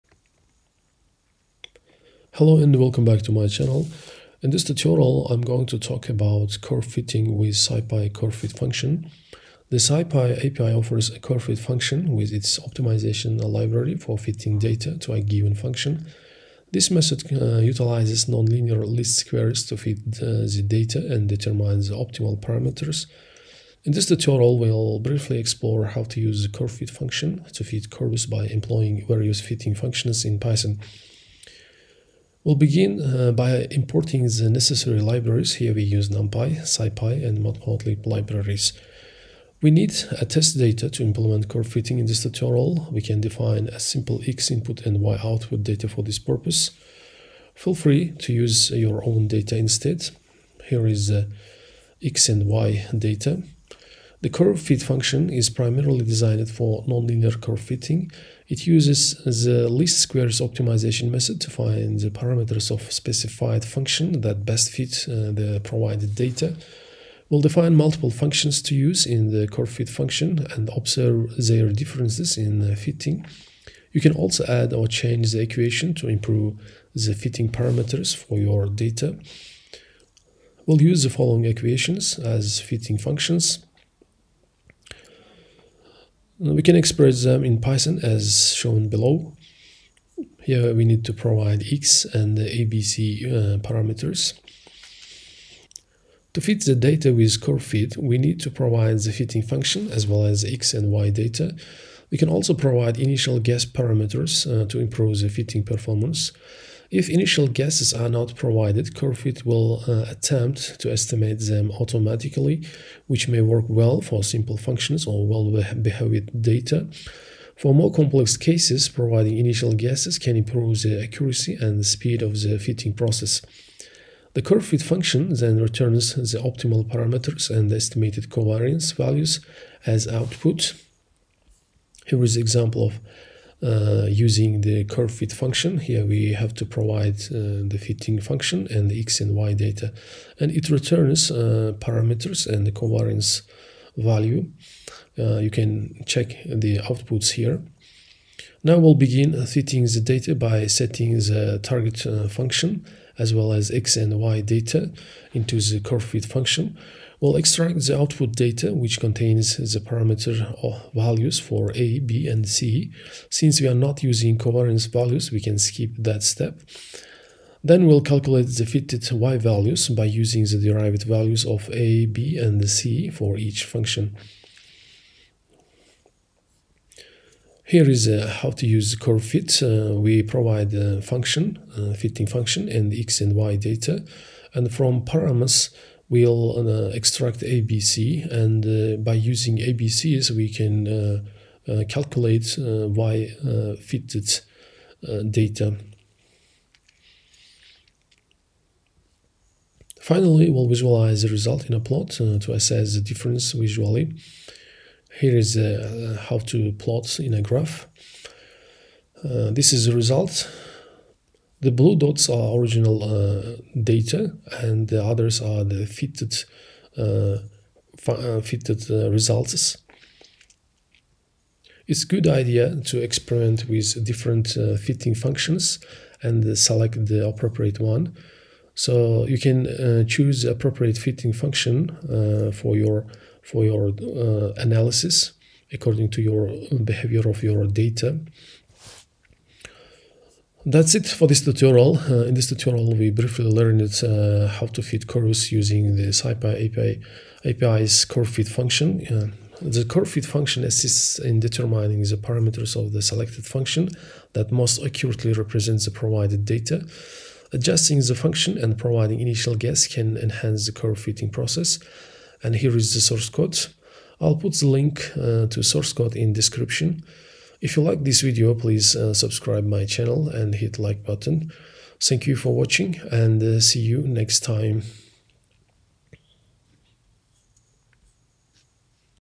Curve fitting with SciPy curve_fit function
In this video, I explain how to use the curve_fit() function to fit curves by employing various fitting functions in Python. The source code for the tutorial is provided below.